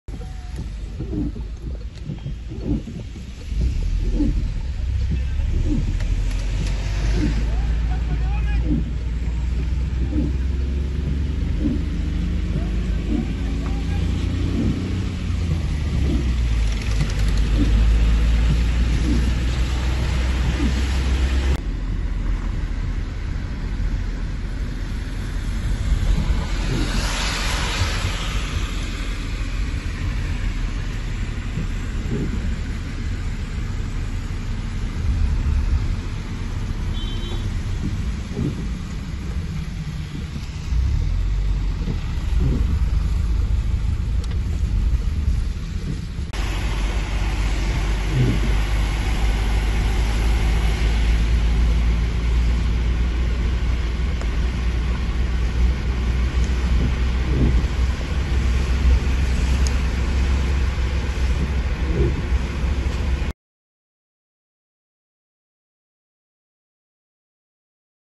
Maraş'tan çıkışlarda uzun kuyruklar
Maraş’ın yakın ilçelerinden Göksun’da  yıkılan binalarda arama kurtarma çalışmaları devam ederken ilçe merkezi boyunca evlerin hemen hepsinde çatlaklar gözlendi. Kar yağışının etkisini artırdığı bölgede Maraş’tan çıkan yüzlerce araç uzun kuyruklar oluşturdu.